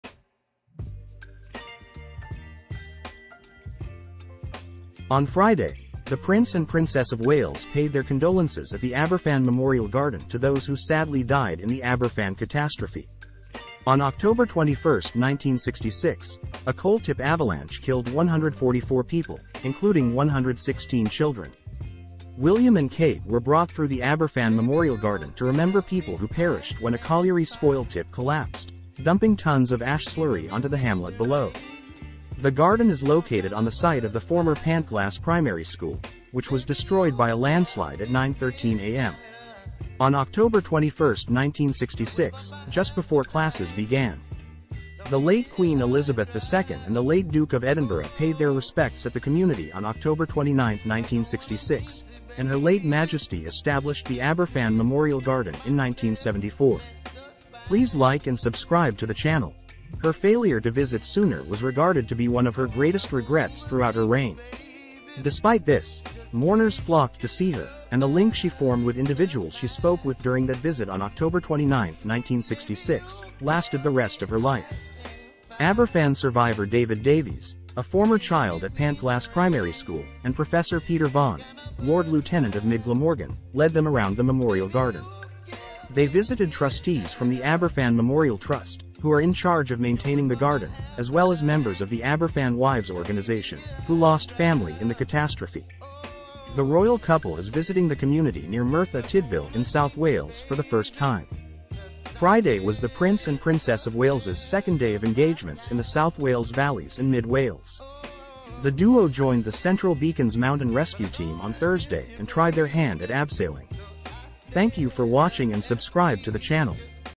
SHOCK NEWS! The Prince and Princess of Wales were led through the Aberfan Memorial Gardens to pay th
On Friday, the Prince and Princess of Wales paid their condolences at the Aberfan Memorial Garden to those who sadly died in the Aberfan catastrophe.
On October 21, 1966, a coal-tip avalanche killed 144 people, including 116 children.
William and Kate were brought through the Aberfan Memorial Garden to remember people who perished when a colliery spoil tip collapsed, dumping tonnes of ash slurry onto the hamlet below.
The garden is located on the site of the former Pantglas Primary School, which was destroyed by a landslide at 9:13 a.m. on October 21, 1966, just before classes began.
The late Queen Elizabeth II and the late Duke of Edinburgh paid their respects at the community on October 29, 1966, and Her Late Majesty established the Aberfan Memorial Garden in 1974.
Her failure to visit sooner was regarded to be one of her greatest regrets throughout her reign.  Despite this, mourners flocked to see her, and the link she formed with individuals she spoke with during that visit on October 29, 1966, lasted the rest of her life.
Aberfan survivor David Davies, a former child at Pantglas Primary School, and Professor Peter Vaughan, Lord Lieutenant of Mid Glamorgan, led them around the memorial garden.
They visited trustees from the Aberfan Memorial Trust, who are in charge of maintaining the garden, as well as members of the Aberfan Wives organization, who lost family in the catastrophe.
The royal couple is visiting the community near Merthyr Tydfil in South Wales for the first time.
Friday was the Prince and Princess of Wales' second day of engagements in the South Wales Valleys and Mid Wales.
The duo joined the Central Beacons Mountain Rescue Team on Thursday and tried their hand at abseiling.